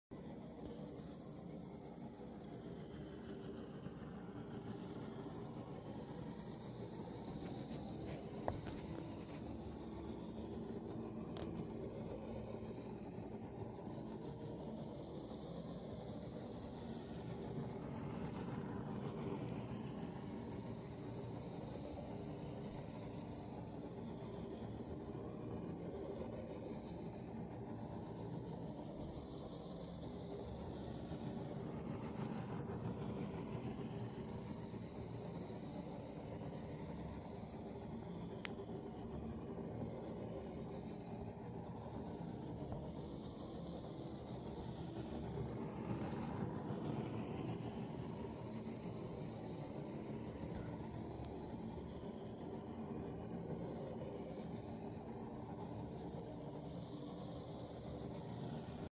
TEASER! Unlock 360° Sisyphus Table View: Be in the Heart of the Action
@StarfishInnovations Here's an exciting challenge for all my viewers! I've created a unique 360° video featuring our mesmerizing Sisyphus sand table, captured with the Insta360 RS-1 camera.

The camera is suspended right in the middle of the table, offering a fully immersive experience that makes you feel like you're part of the action, with sand patterns and balls rotating around you.

🌀🎥 If this teaser video gets 50 likes, I'll release the full 360° view in stunning 4K resolution. It’s an incredible opportunity to explore the intricate movements of the Sisyphus table as if you were sitting right in its center.

This video is also VR compatible, offering an even more immersive experience